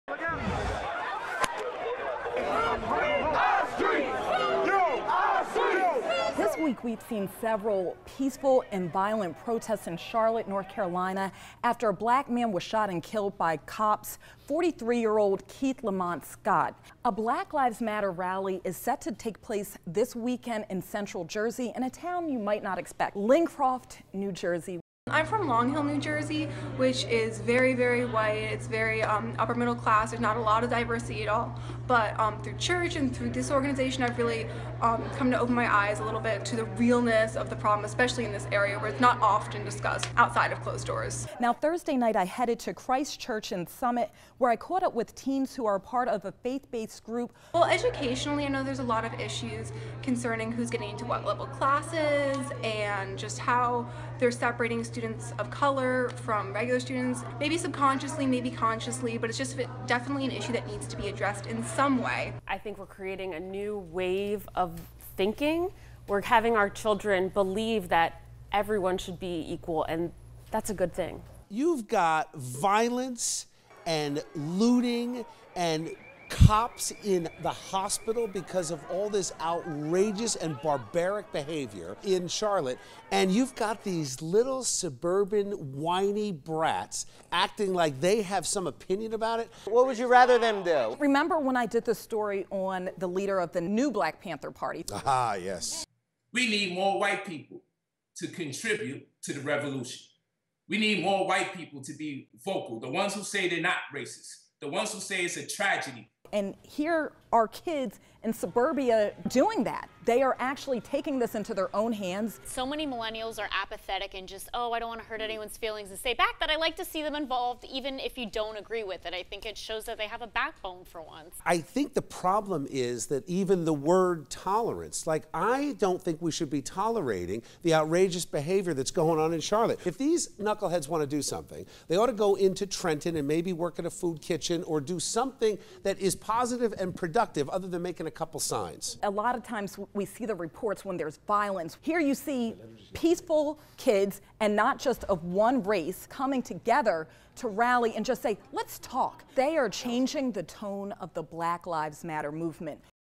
Ivory For Ebony
The Universalist Unitarian Congregation of Monmouth County in Lincroft, NJ is holding a Black Lives Matter rally Sunday at 1:30 p.m. Ashley headed to Christ Church in Summit where she caught up with teenagers who belong to the STAR -- Standing Together Against Racism. They are participating in the rally and were making banners.

Whitney Porter, a senior at Wardlaw-Hartridge High School in Edison, talked about the need for such an event like this and shared experiencing racism at a young age.

She said she remembered "certain things like 'you're not pretty because you're black,'" Whitney said. She said  it made her feel "upset and bad about myself."

The youth organization is diverse and made up of many white children who are participating in the Black Lives Matter march.